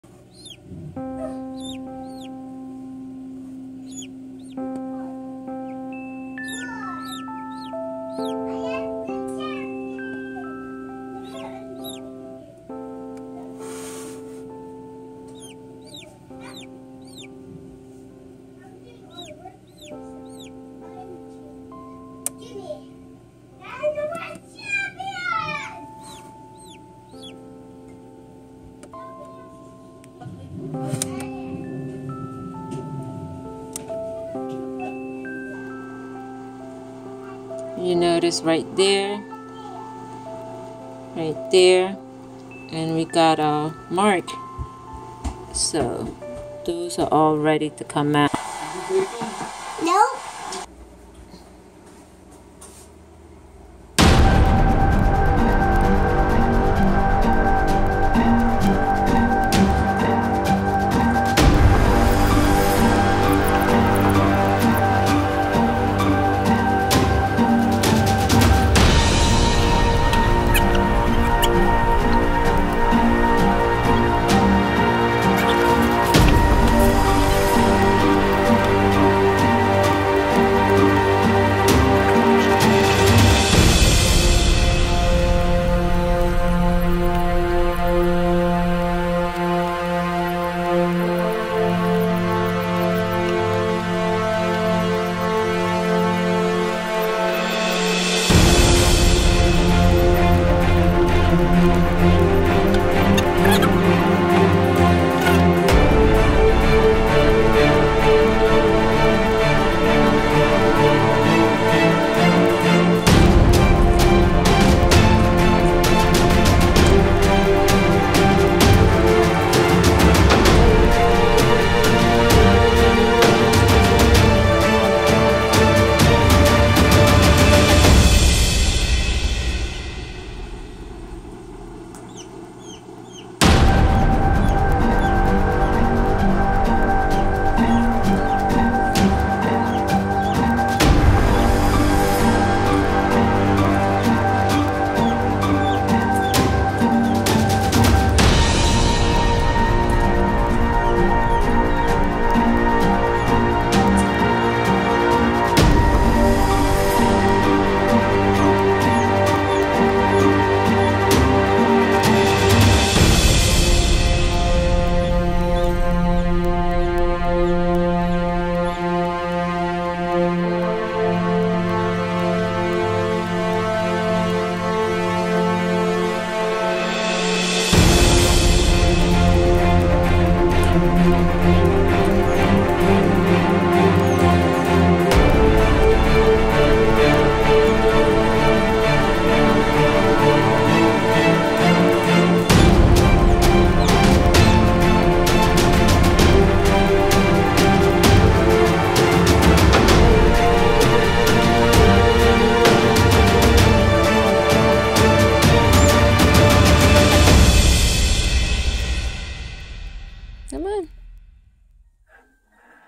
How to Never Help Chick Hatch !
A healthy hatching innately knows exactly what to do, and you shouldn’t interfere.

Is it worth it to help chicks who are having trouble getting out of the shell?  For that matter, how can you tell if they're having
trouble?  If you do help, how? For me No I wont help because I am sad to see it die couple hours later.